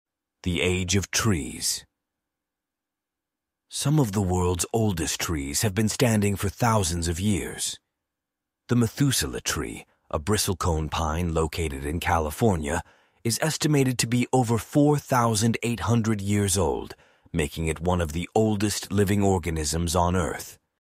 The Age of Trees#AncientWisdom
The Ageless Sentinels of Time: Exploring the Ancient Trees of Our World"

Amidst the bustling modern world, ancient giants stand as silent witnesses to the passage of time. These arboreal marvels, known as the Methuselahs of the plant kingdom, bear the weight of centuries on their weathered shoulders. Among them, the Methuselah tree, a bristlecone pine, emerges as a true living relic, having silently observed the rise and fall of civilizations for over 4,800 years.